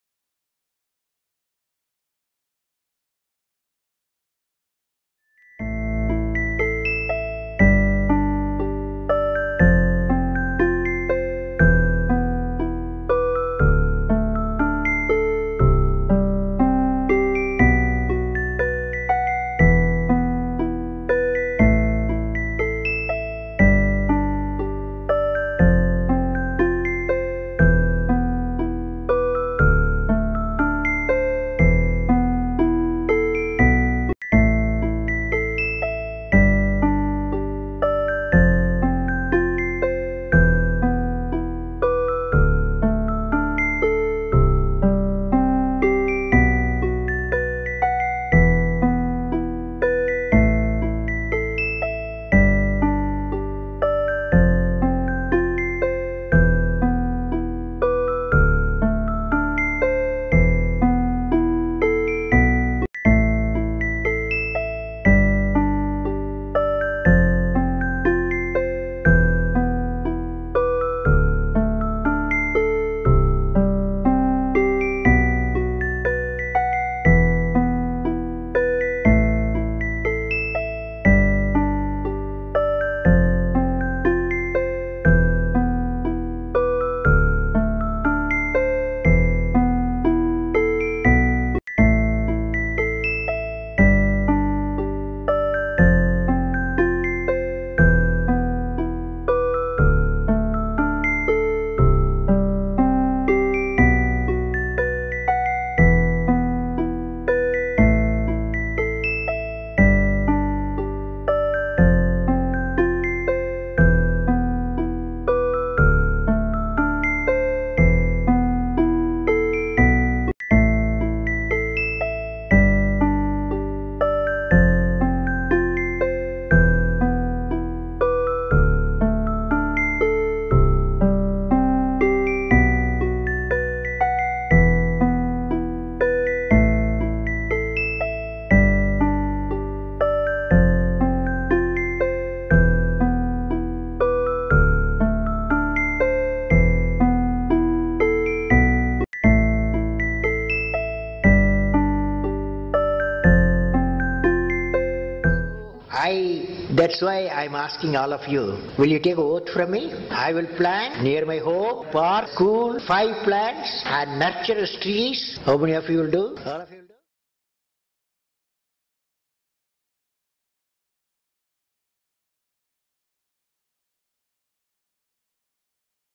Unknown Facts about Dr.A.P.J.Abdul Kalam; A Small Tribute To the MissileMan Of India
Dr. APJ Abdul Kalam (Avul Pakir Jainulabdeen Abdul Kalam) the 11th President of India from 2002 to 2007, was born in Rameshwaram, Tamil Nadu on October 15, 1931. An aerospace scientist, who spent the majority of his life at the Defence Research and Development Organisation (DRDO) and Indian Space Research Organisation (ISRO), APJ Abdul Kalam was intimately involved in the nation's civilian space program and military missile development efforts.